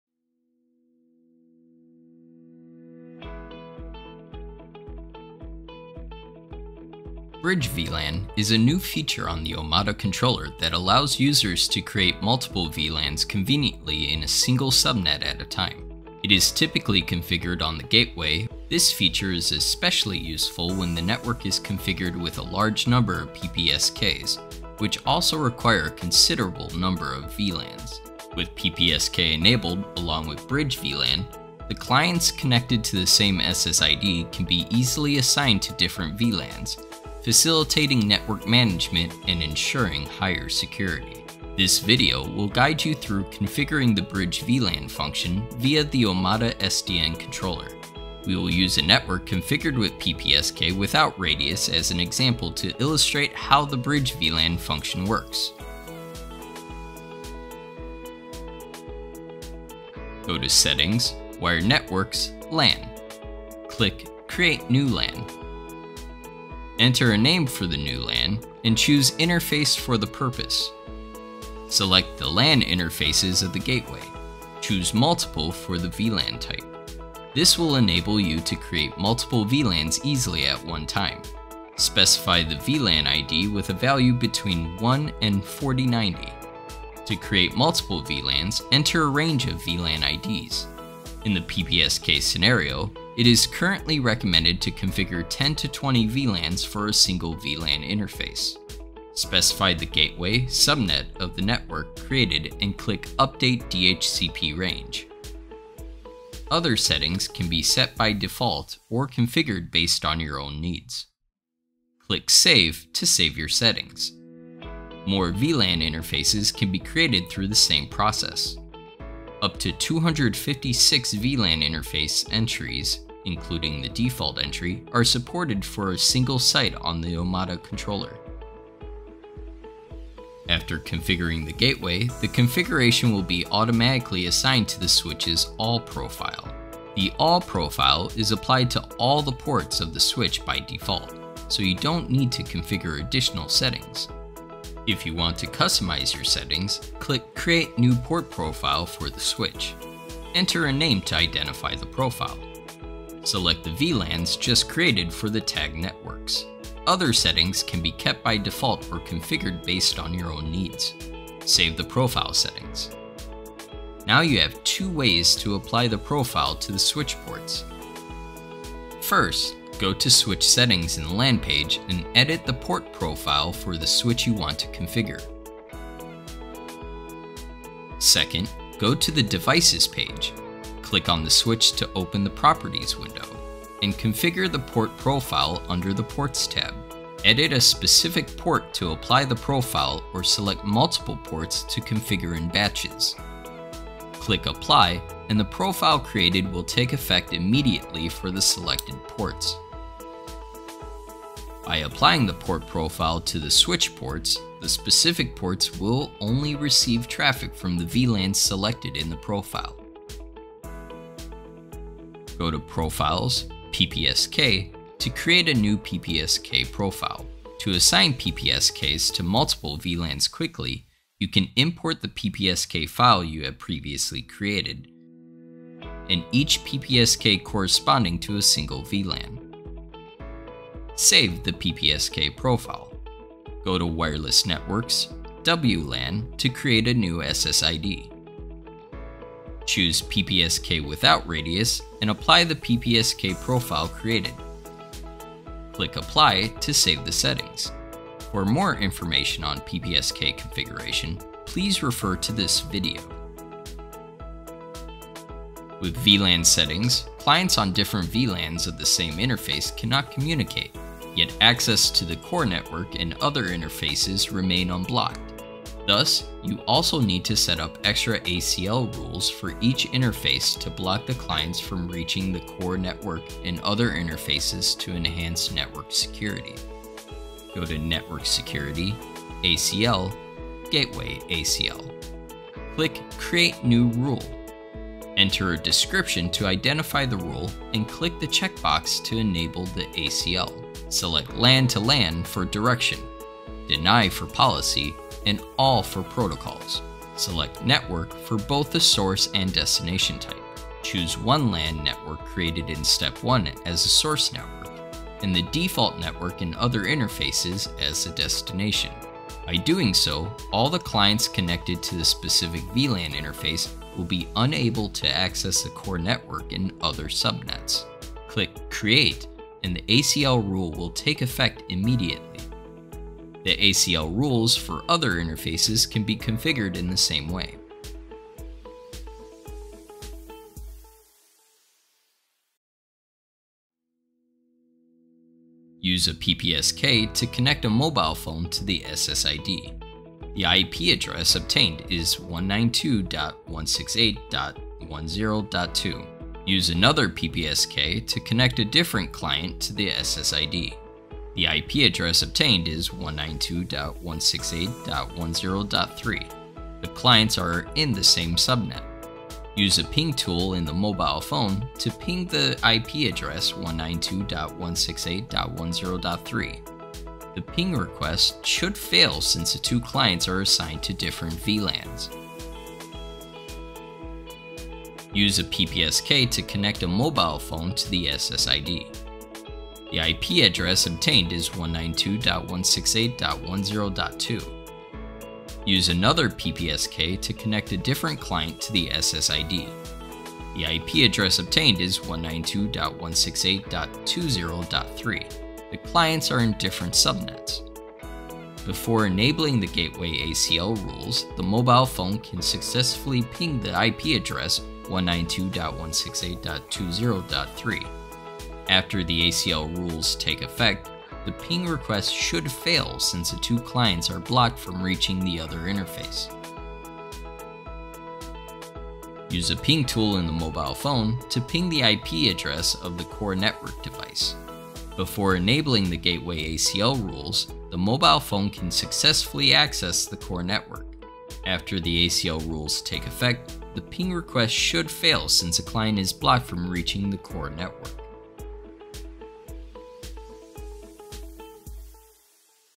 How to: Configure Bridge VLAN for PPSK via the Omada SDN Controller
This video will show you how to configure bridge VLAN for PPSK via the Omada SDN Controller.

Timestamps：
00:00:57 Part 1 Configuration
 00:00:58 Step 1 Configure the Gateway
 00:02:09 Step 2 Configure the Switch (Optional)
 00:03:31 Step 3 Configure the AP
 00:04:14 Step 4 Configure Gateway ACL
00:05:33 Part 2 Test